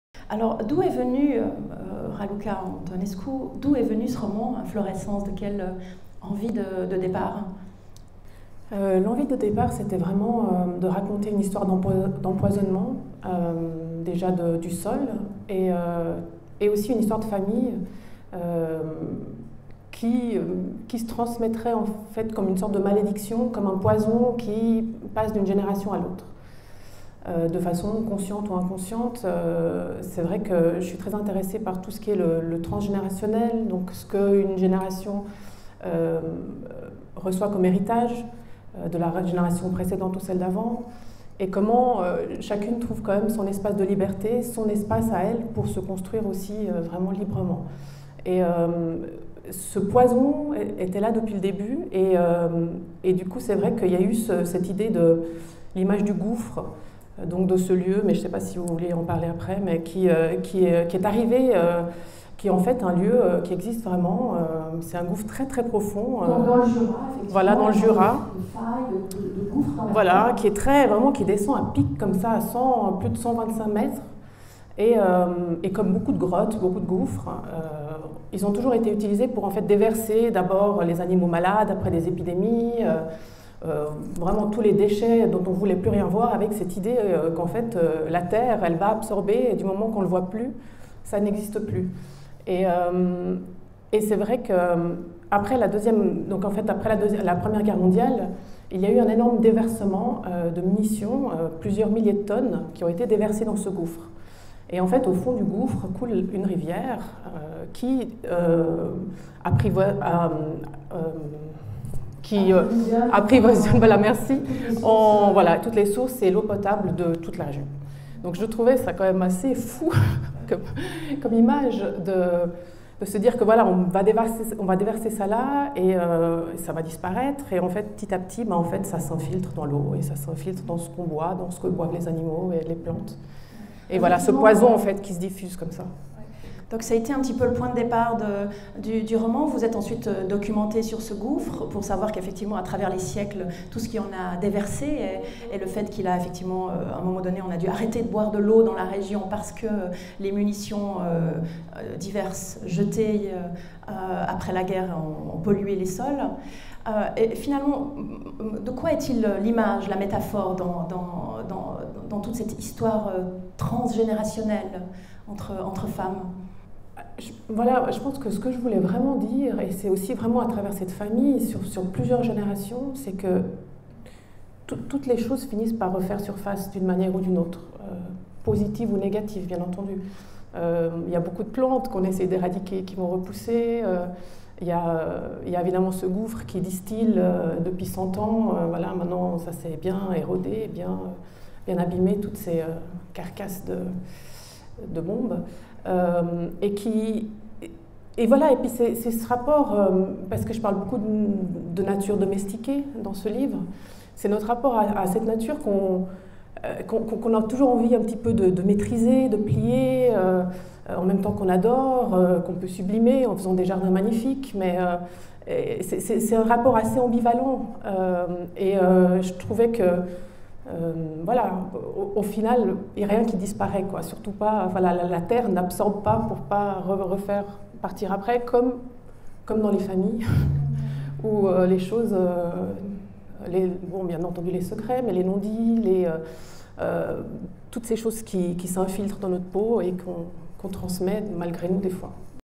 Rencontre avec l’écrivaine Raluca Antonescu - Prix des lecteurs de la Ville de Lausanne 2022
L’écrivaine genevoise Raluca Antonescu, en lice pour le Prix des lecteurs de la Ville de Lausanne 2022 avec son roman «Inflorescence» (éditions La Baconnière), raconte son parcours, ses sources d’inspiration et son lien à la nature au micro d’Isabelle Falconnier, déléguée à la politique du livre, le samedi 2 octobre 2021 au Lausanne Palace.